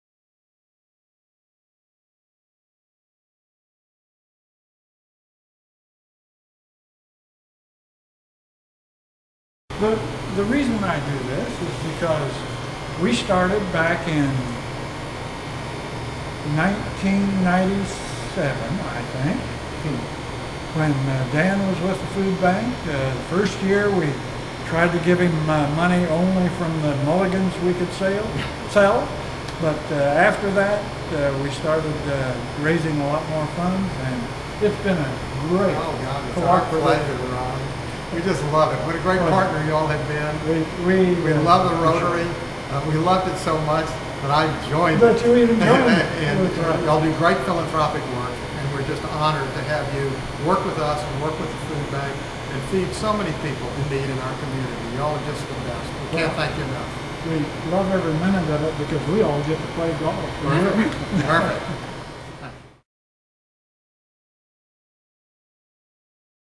Ron Lantz Helps MOWAM
Every year, the University Area Rotary Club & Lantz Invitational Charity Golf Tournament is held in Austin, Texas to benefit Meals on Wheels and More and the Capital Area Food Bank. In this video, Ron Lantz explains why he chose MOWAM as a beneficiary.